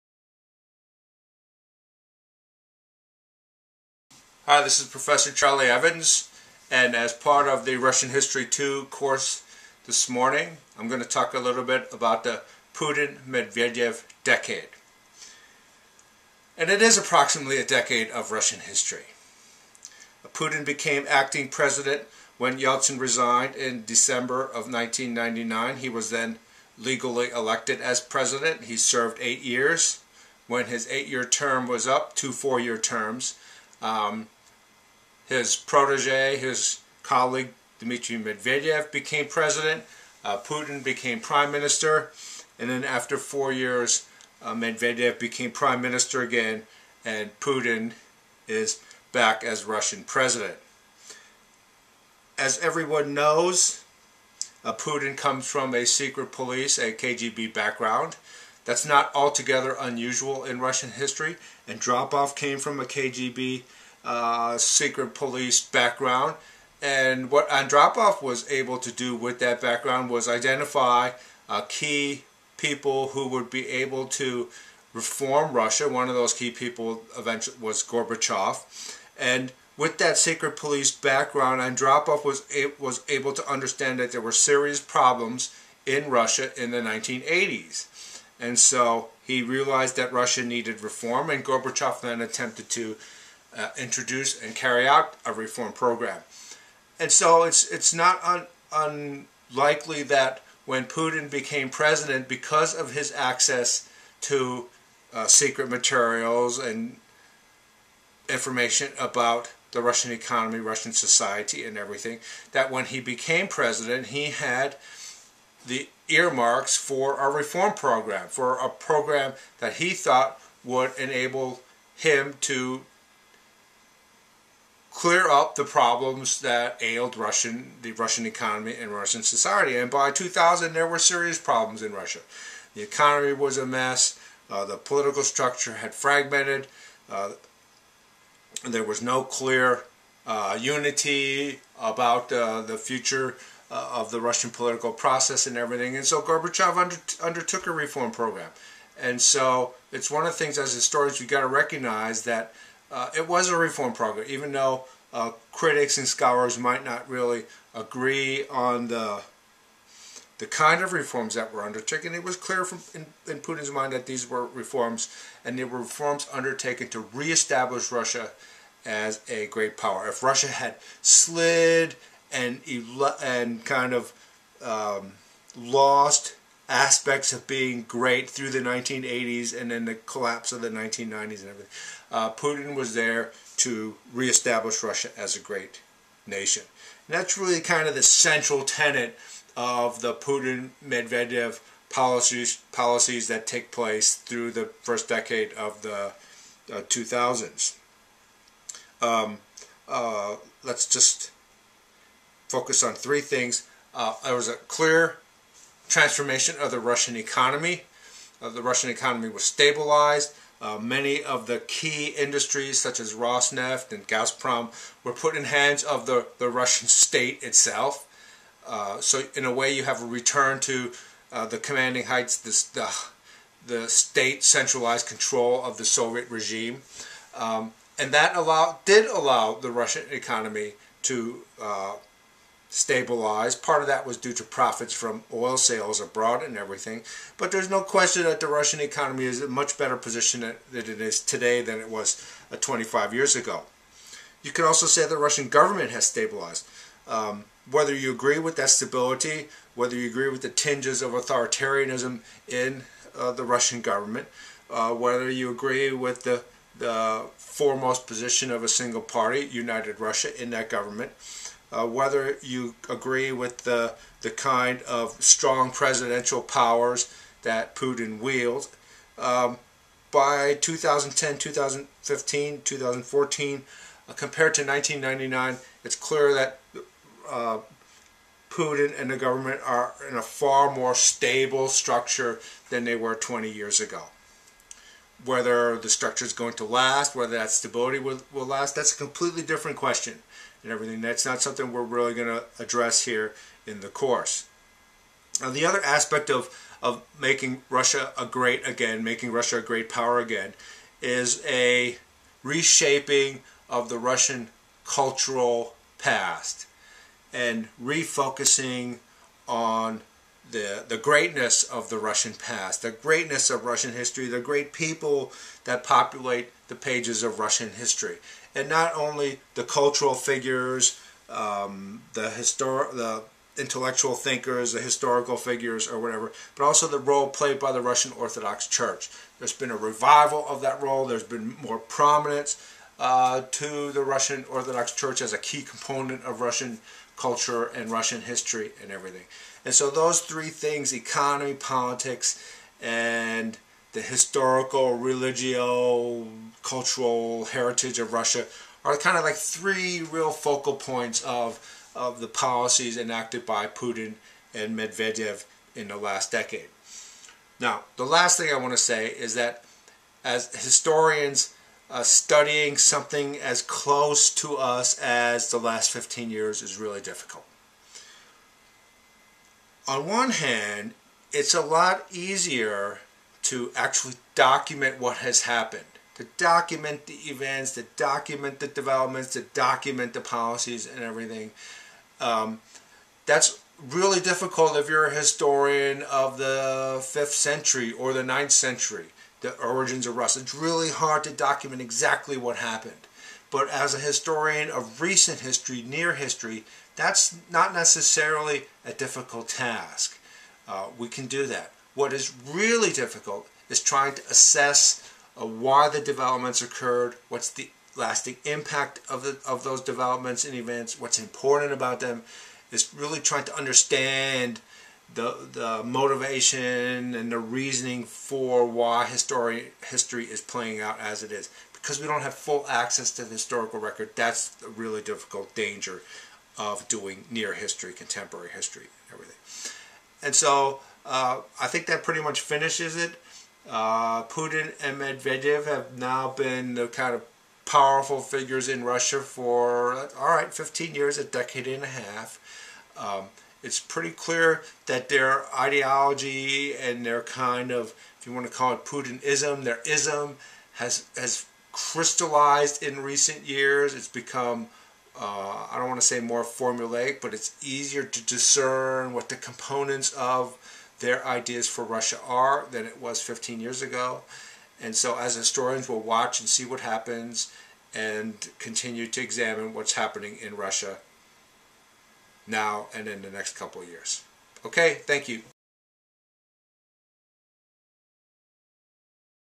Putin and Medvedev: Some Real Quick Remarks
Short online lecture to support the Russian history II course.